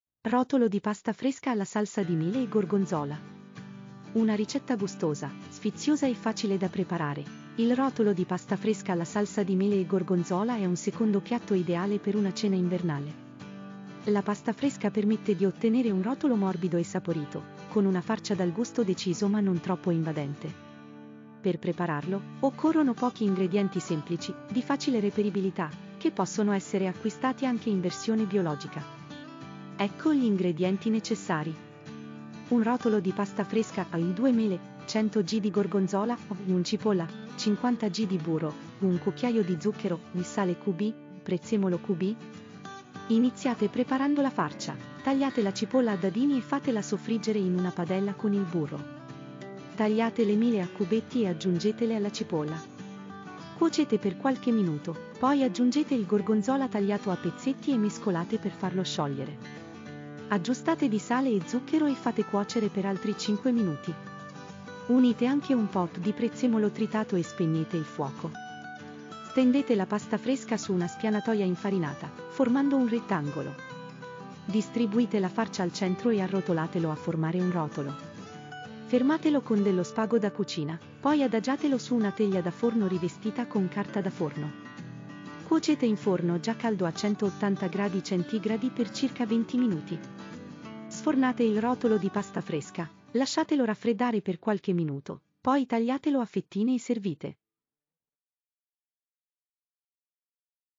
Rotolo di pasta fresca alla salsa di mele e gorgonzola
Un rotolo di pasta fresca al forno con una golosa farcia a base di mele e gorgonzola, la ricetta perfetta per una cena invernale! - Scopri di più su